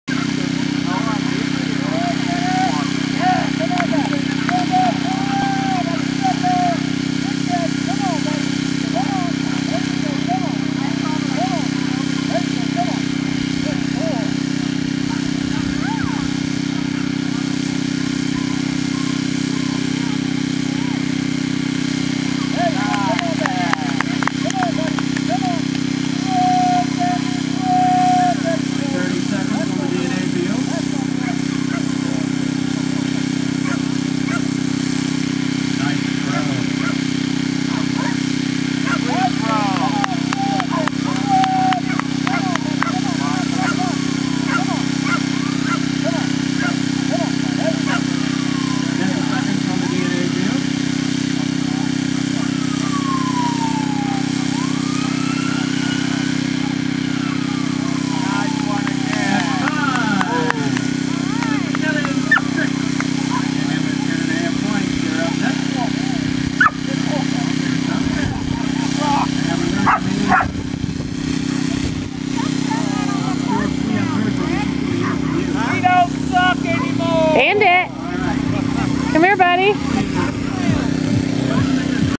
2008 SLCQ Chris Engel and Bandit (DA Round2)
2008 Skyhoundz Last Chance Qualifier

CFDDC Member Chris Engel competes for a chance at this years Skyhoundz world final competition in TN

Chris took 2nd overall for Novice Division.
Chris took 18th overall for Expert Division.

for more videos, pictures and information to learn how you can become tomorrows world champion

Webmaster: Chris Engel